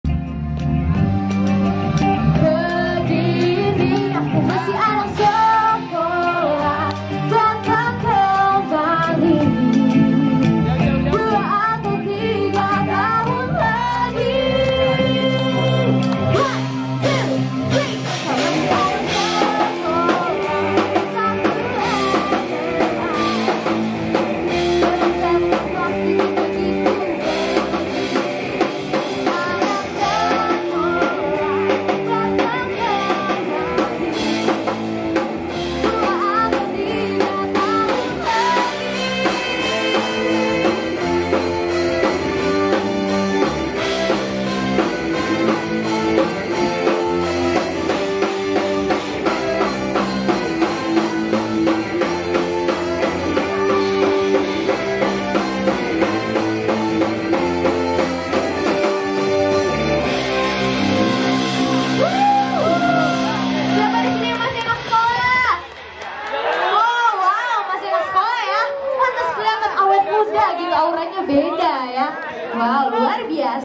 Gitaris Sahitya Band, Beraksi di Depan Kader Gerindra Se Tangerang Raya
Pertunjukan ini digelar saat Gerindra Kota Tangerang menggelar Sepeda Sehat dan Senam Gembira di Lapanag Jend. Ahmad Yani atau Alun-alun Kota Tangerang